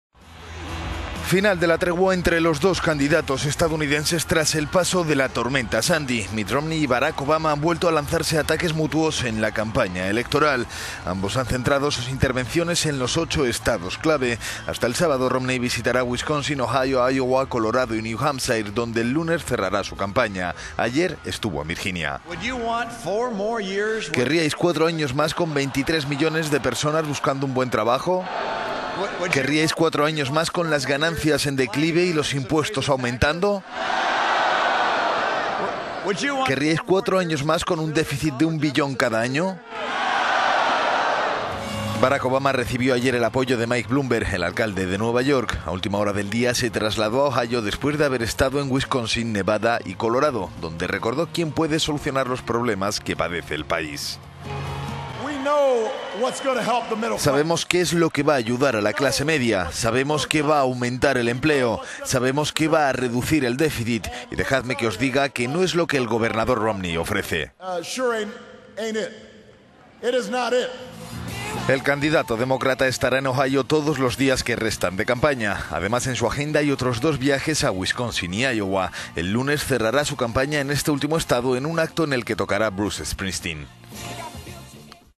Romney y Obama ponen fin a la tregua por la tormenta Sandy
Tras el paso de la tormenta Sandy, la campaña estadounidense vuelve a endurecerse. Mitt Romney y Barack Obama han vuelto a lanzarse dardos mutuos.

Ambos han centrado sus intervenciones en los ocho Estados clave. Hasta el sábado Romney visitará Wisconsin, Ohio, Iowa, Colorado y New Hampshire, donde el lunes cerrará su campaña. Ayer estuvo en Virginia preguntando a sus seguidores.

 "¿Querríais cuatro años más con 23 millones de personas buscando un buen trabajo?¿Querríais cuatro años más con las ganancias en declive y los impuestos aumentado?¿Querríais cuatro años más con un déficit de un billón cada año?"

Barack Obama recibió ayer el apoyo de Michael Bloomberg, el alcalde de Nueva York. A última hora del día se traslado a Ohio, después de haber estado en Wisconsin, Nevada y Colorado. donde recordó quién puede solucionar los problemas que padece el país.

"Sabemos qué es lo que va a ayudar a la clase media, sabemos qué va a aumentar el empleo, sabemos qué va a reducir el déficit y dejadme que os diga que no es lo que el gobernador Romney ofrece."

El candidato demócrata estará en Ohio todos los días que restan de campaña. Además en su agenda hay otros dos viajes a Wisconsin y Iowa. El lunes cerrará su campaña en este último Estado en un acto en el que participará Bruce Springsteen.